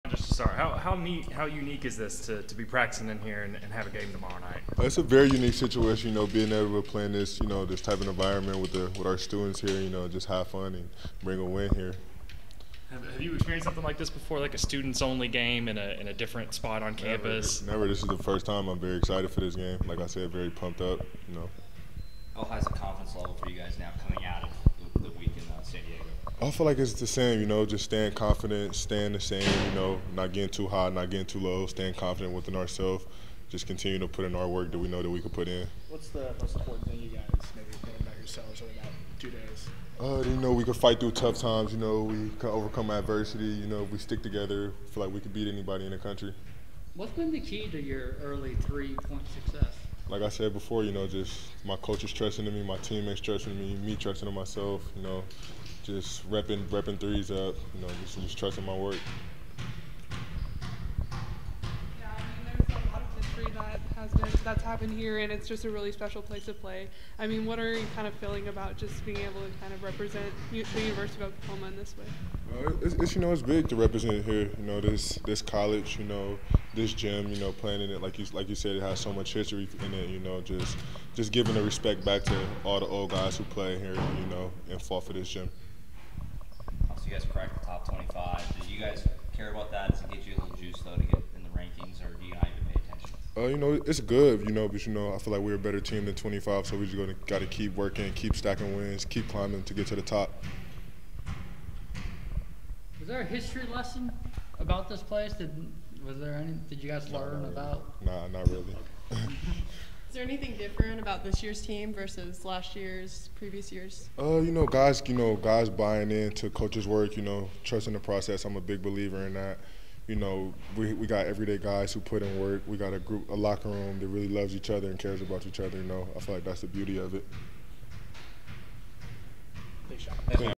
11/29/2023 Oklahoma F/C John Hugley interview (Pre-Ark PB)
Oklahoma forward/center John Hugley met with the media before practice on Wednesday, November 29th ahead of the Sooners' matchup with Arkansas Pine-Bluff at McCasland Fieldhouse on Thursday night in Norman.